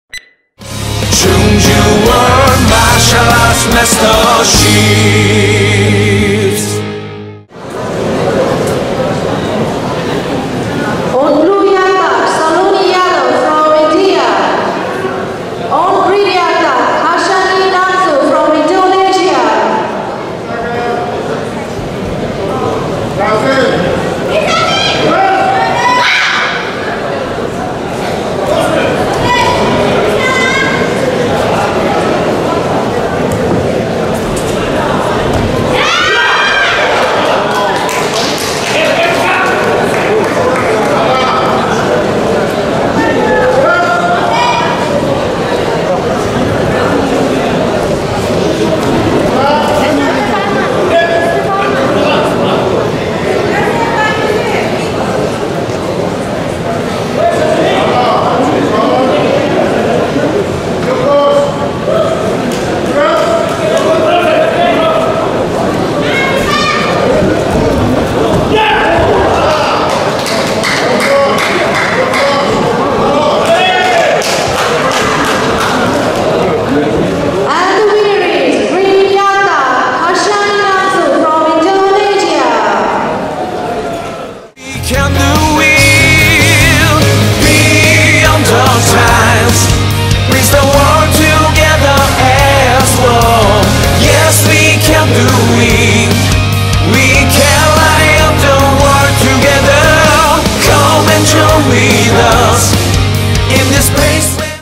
Saloni Yadav 🆚 Khasani Najmu Shifa l 2019 World Martial Arts Masterships l Kurash -63kg
BSC Media
[2019 Chungju World Martial Arts Masterships Kurash & The 12th(XII) World Kurash Senior Championships : Saloni Yadav(India) vs Khasani Najmu Shifa(Indonesia) - 3rd Sep 2019 Tuesday

This match is Female -63kg of Kurash of '2019 Chungju World Martial Arts Masterships' & 'The 12th(XII) World Kurash Senior Championships' in Korea National University of Transportation Chungju Campus gymnasium.